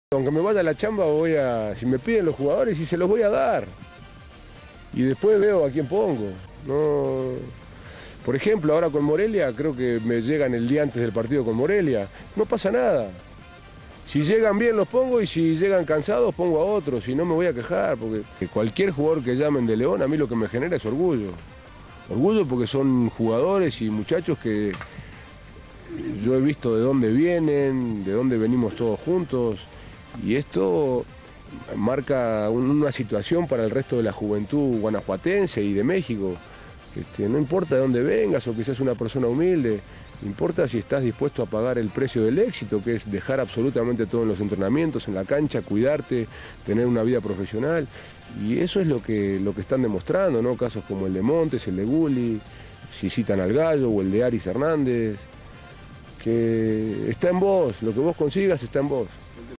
Gustavo Matosas habla de la selección mexicana
Gustavo Matosas, entrenador del León, habla de los seleccionados de su equipo y de cómo busca apoyarlos cuando jueguen con el Tricolor aunque después tenga que checar con quién jugará en la Liga Bancomer Mx.